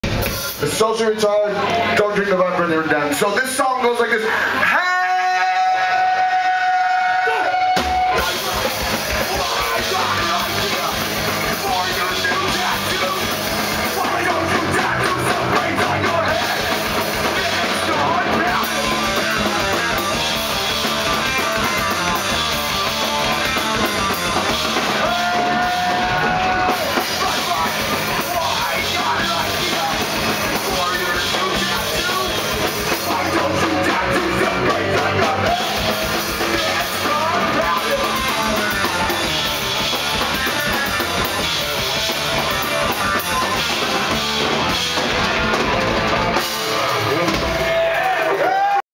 Scheissse Minnelli - Live - Cologne, Germany.
Scheissse Minnelli at MTC. Cologne, Germany. November 5th, 2011. Opening for DRI.